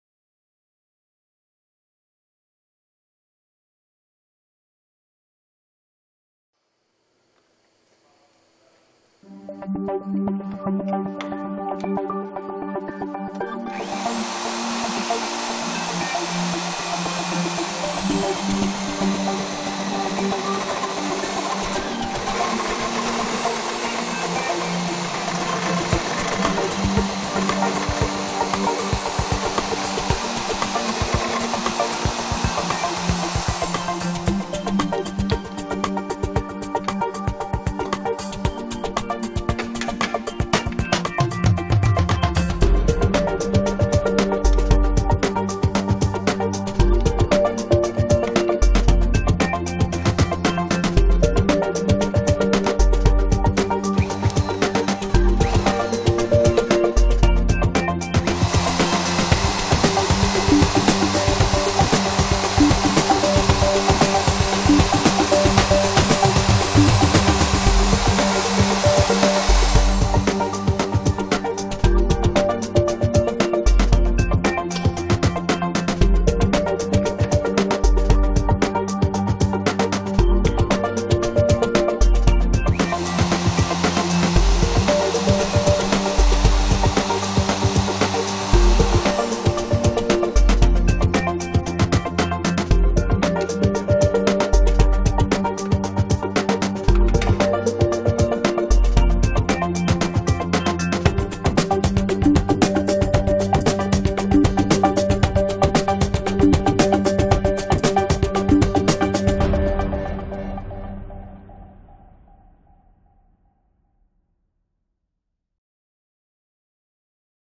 T-DRILL HFT-2000 SS Collaring unit 2017
T-DRILL HFT-2000 SS is a manually operated unit that is used
in conjunction with the T-DRILL T-65 SS collaring machine in
small volume production of stainless steel manifolds.

Hand Feed Table is capable of producing collars of 17–51 mm in run tubes of 32–114mm. With the HFT-2000 SS you can produce five 51 mm collars in less than ten minutes without moving the tube!